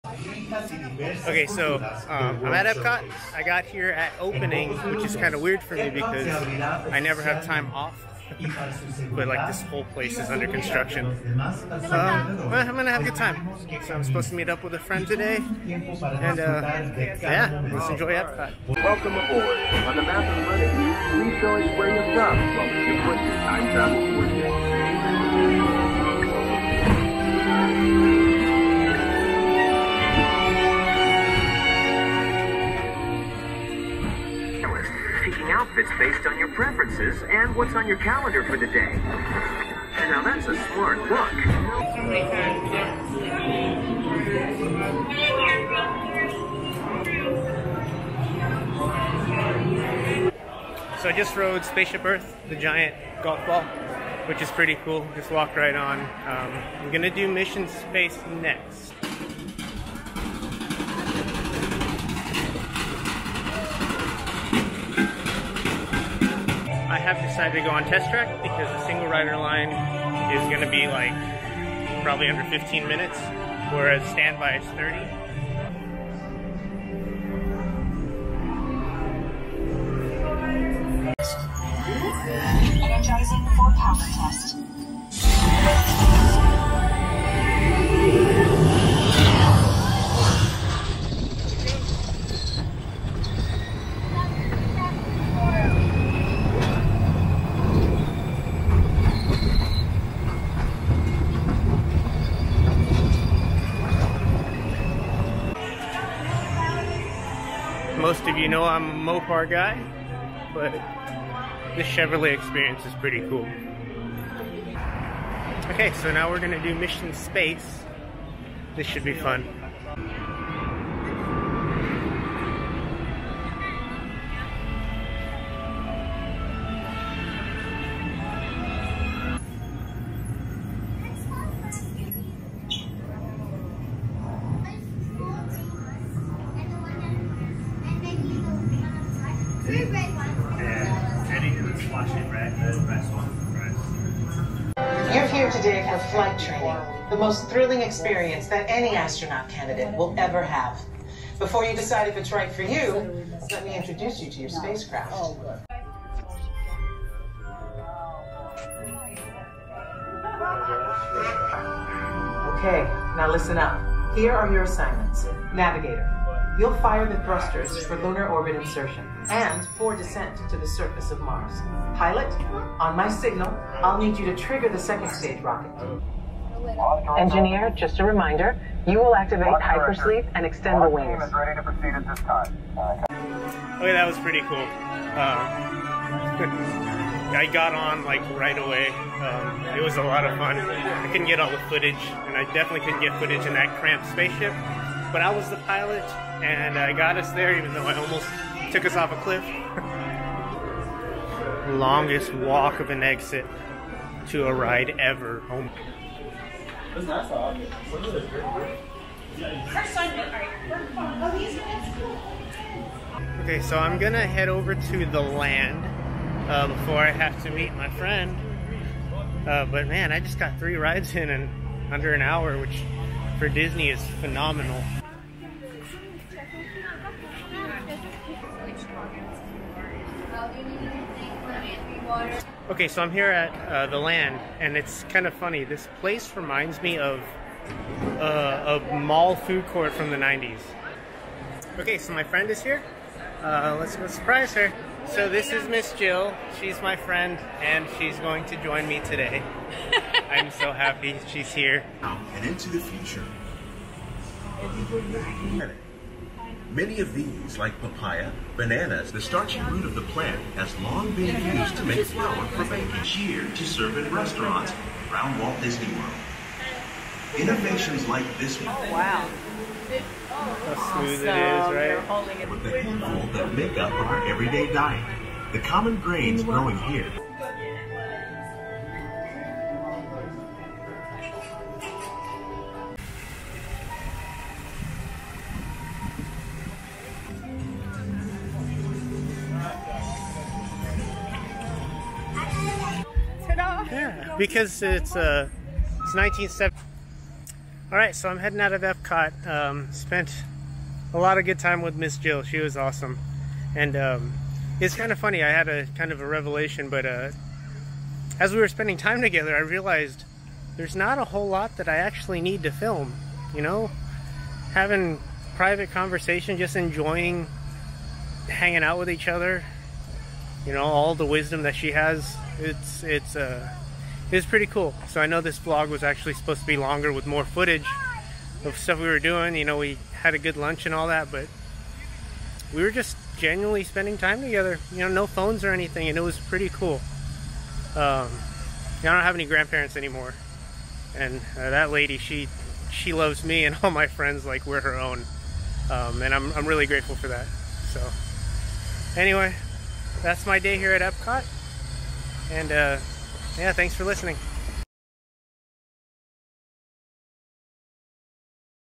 A Day at Epcot
Had a day off, so I wanted to spend it at Epcot, and even got to meet up with my friend, Ms. Jill!